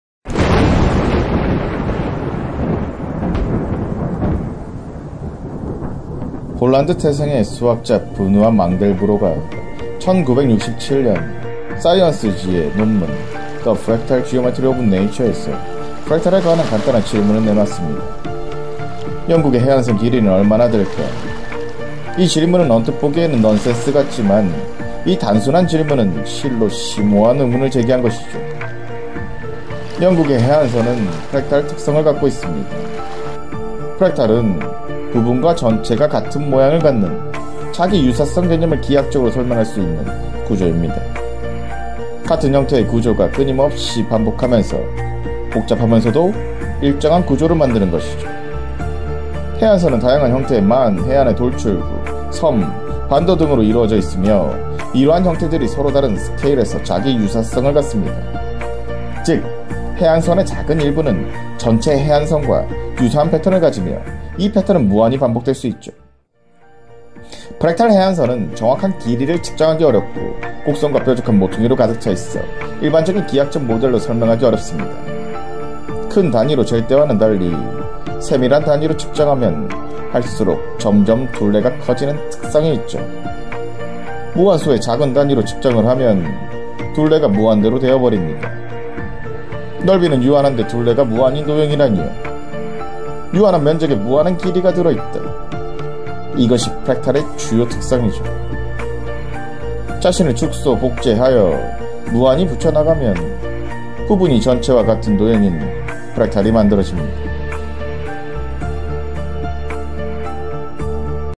영국 땅 크기는 정해져 있는데, 둘레는 무한대라고? 🤯
지도의 해안선 길이를 자로 잴 수 있을까?
안녕, 무한의 세계로 안내할 강태공 쌤이야! 🐾

"면적은 유한한데, 둘레는 무한하다."
말이 안 되는 소리 같지?
하지만 수학자 만델브로는 이걸 증명해 냈어.

부분이 전체를 닮고, 그 부분이 또 전체를 닮는
끝없이 반복되는 '자기 유사성(Self-similarity)'의 세계!
자연 속에 숨어 있는 이 기이하고 아름다운
'프랙탈(Fractal)' 구조에 대해 2분 만에 알려줄게.

보고 나면 브로콜리도 다르게 보일걸? 🥦